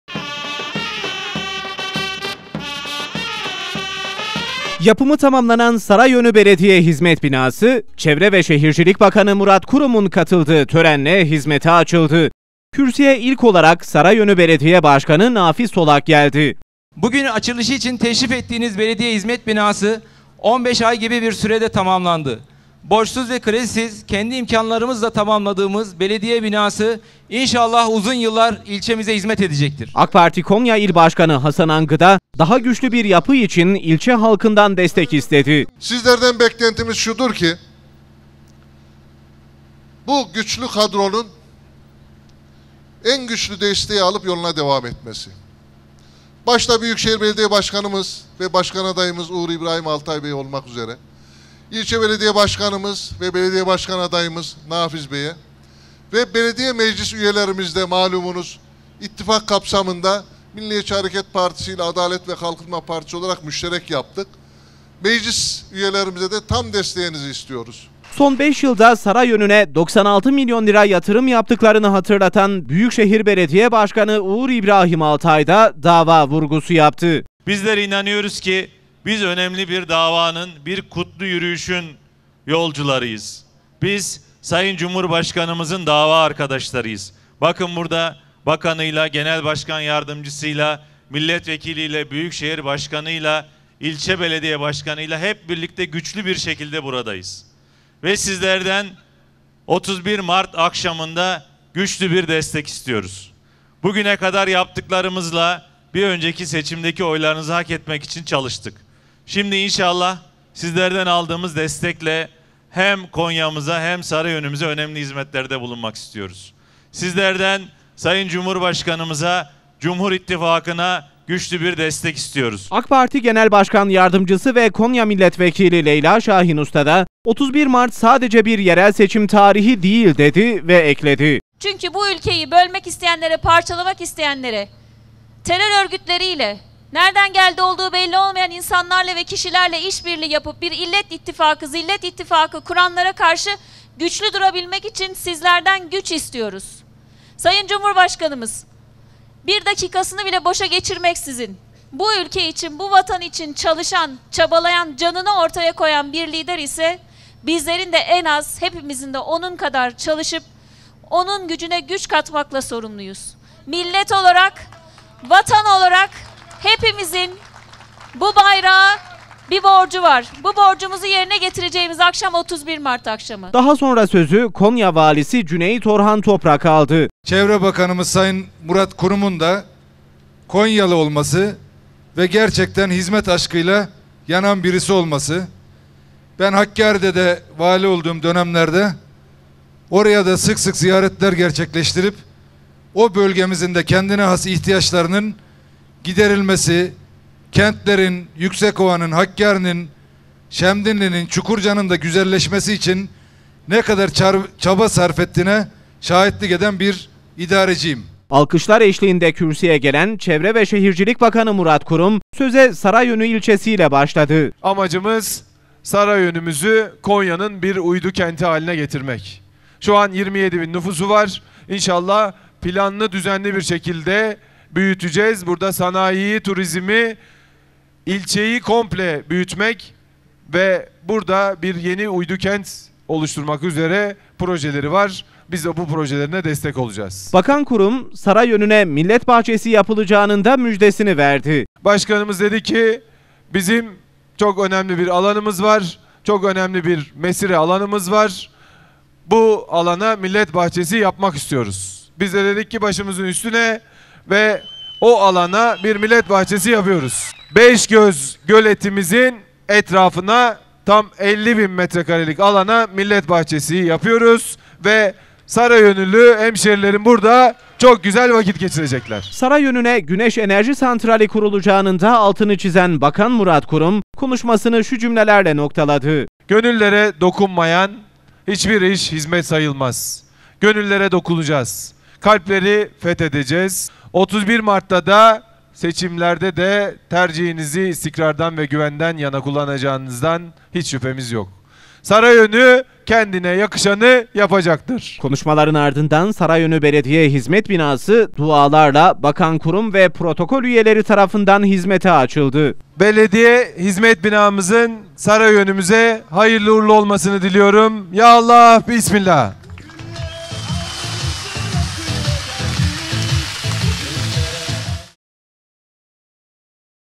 Murat Kurum'dan Sarayönü'ye müjde!
Çevre ve Şehircilik Bakanı Murat Kurum, Sarayönü ilçesindeki açılış töreninde konuşma yaparak Cumhur İttifakına destek istedi.Murat Kurum, ilçeye Millet Bahçesi ve güneş enerji panelini yapılacağını aktardı.